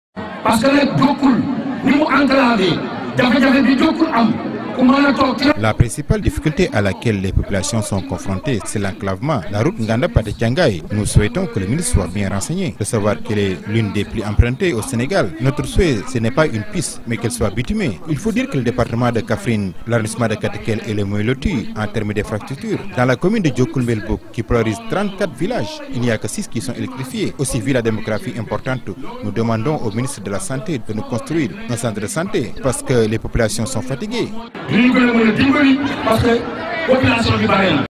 Diockoul Mbelbouck dans le.département de Kaffrone:5 postes de santé pour 32000 habitants,
Thienal fm thiénérou askan wi leeru ndoucoume